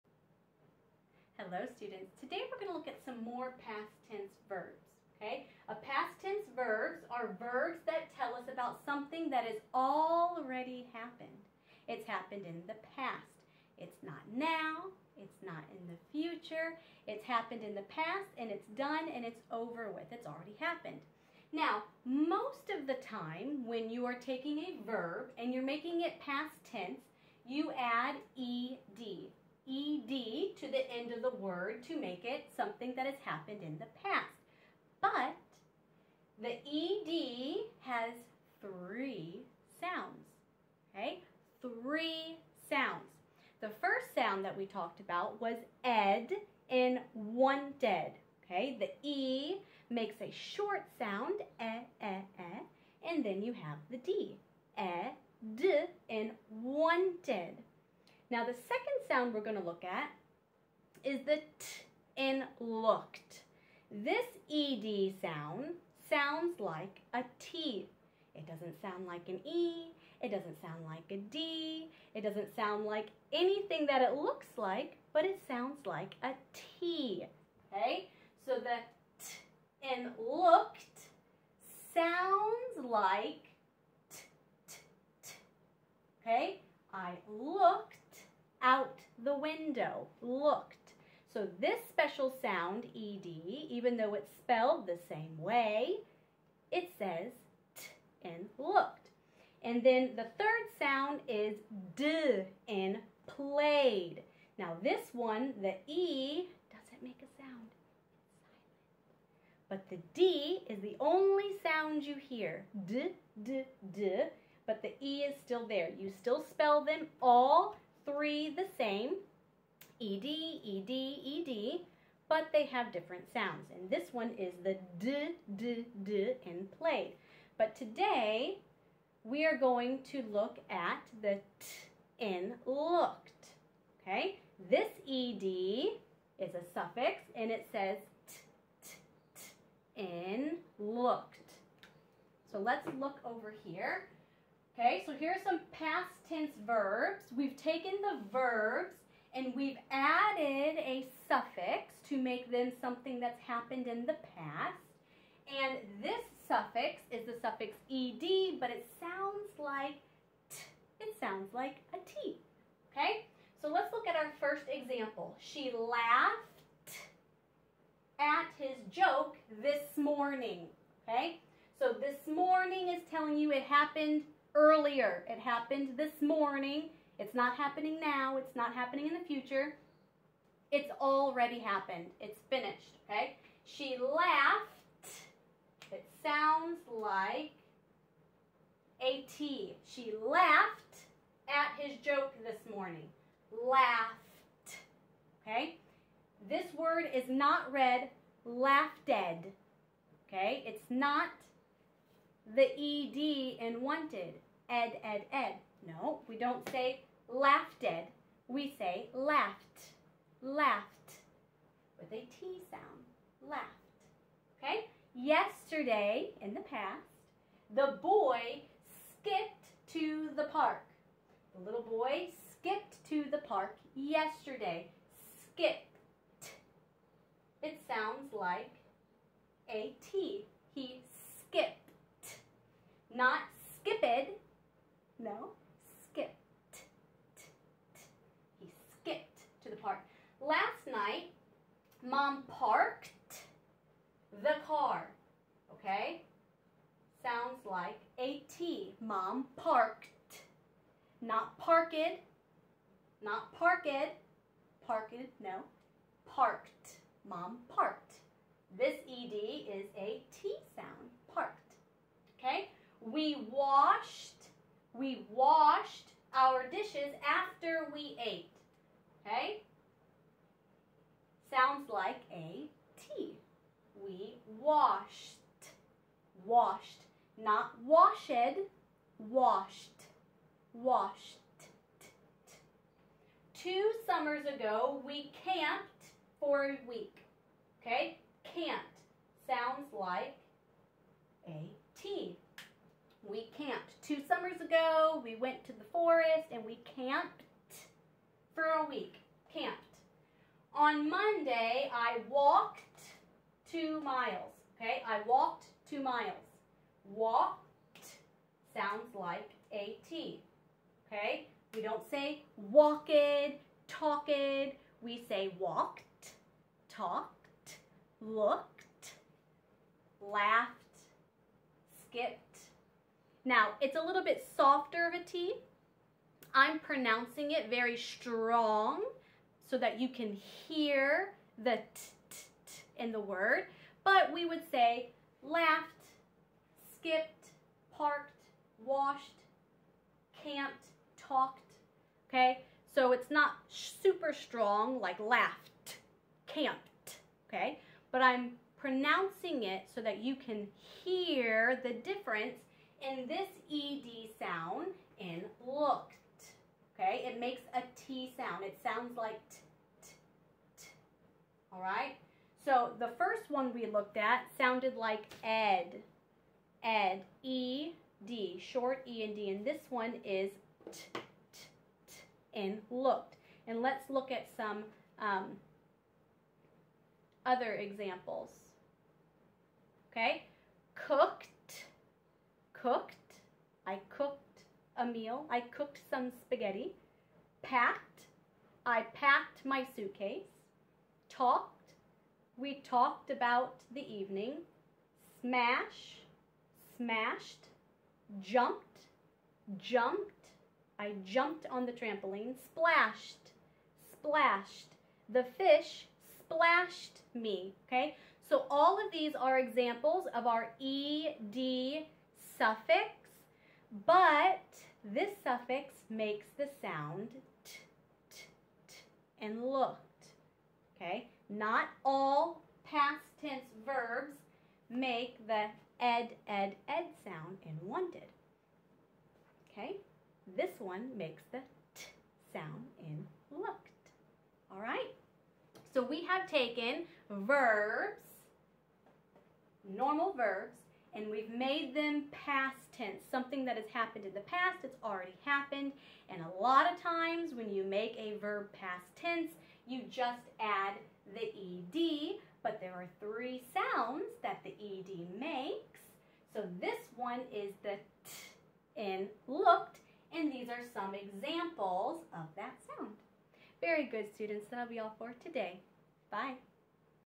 PAST TENSE VERBS: -ED in lookED (makes "t" sound)
When changing a verb to the past tense you normally add the suffix -ed to the end of the root word to make it past tense. However, the suffix -ed actually makes 3 sounds. Which sound does the -ed at the end of the verb make? In this video playlist you will learn the 3 sounds, see examples of how they are used and how to pronounce them correctly.

In this second video the -ed sound is the -ed in looked. The "ed" in this special sound doesn't make either to "e" or "d" sound. It only makes the "t" sound.

Ex.

hook hooked ( 👂🏻 sounds like hook"t" )
laugh laughed ( 👂🏻 sounds like laugh"t" )

Galatians 5:9

A little leaven leaveneth the whole lump.